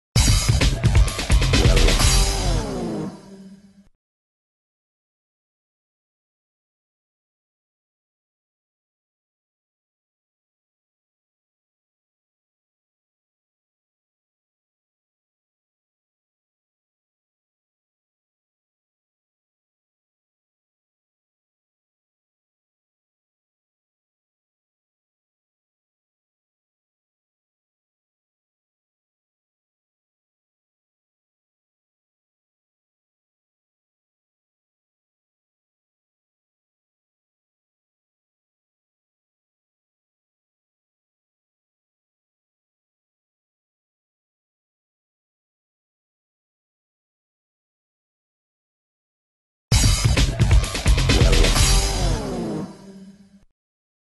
Ladrões invadem loja e ameaçam reféns em SP
Dois bandidos se passam por clientes e anunciam roubo em estabelecimento na zona sul de SP